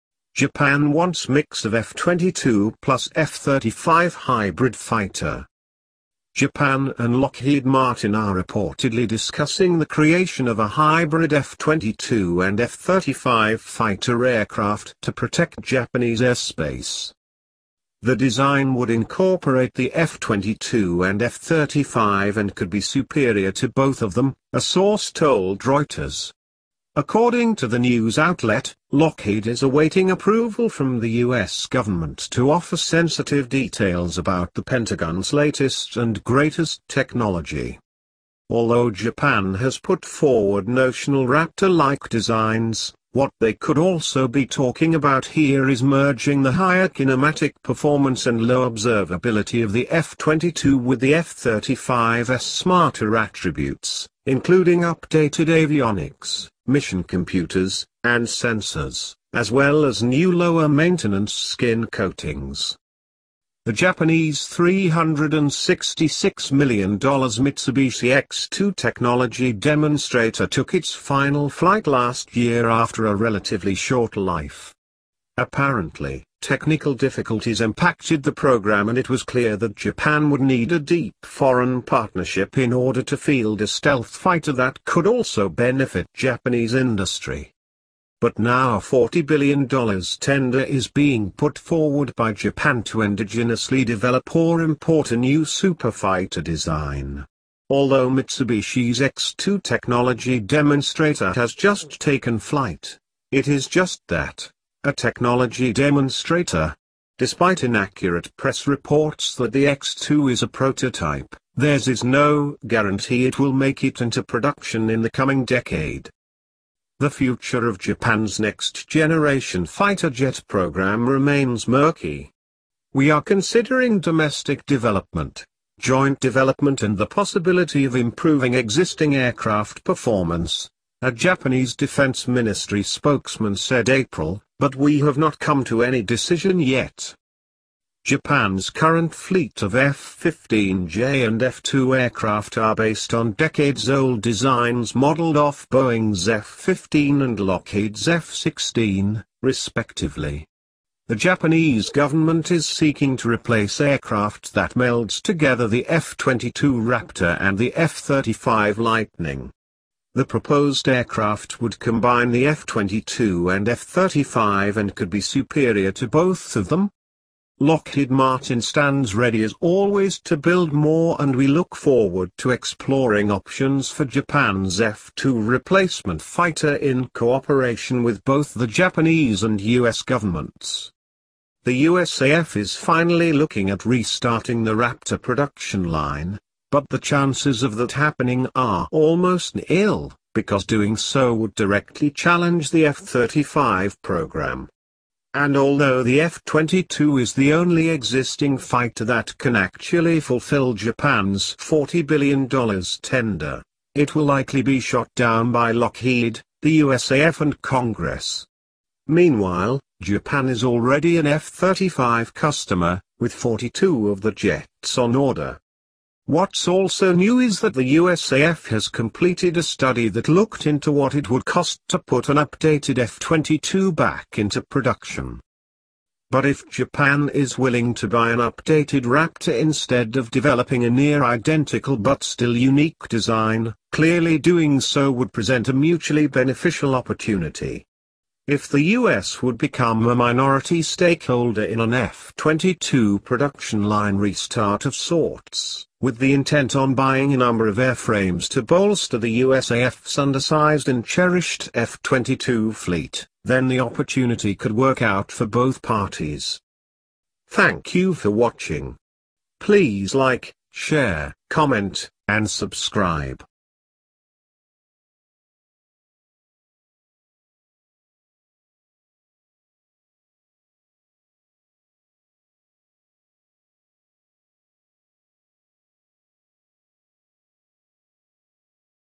Lockheed and Japan to Create Mix of F-22 + F-35 Hybrid Fighter to Japan
Please Support As and Give me a Feed Back to Improve. Japan and Lockheed Martin are reportedly discussing the creation of a hybrid F-22 and F-35 fighter aircraft to protect Japanese airspace. The proposed aircraft “would combine the F-22 and F-35 and could be superior to both of them.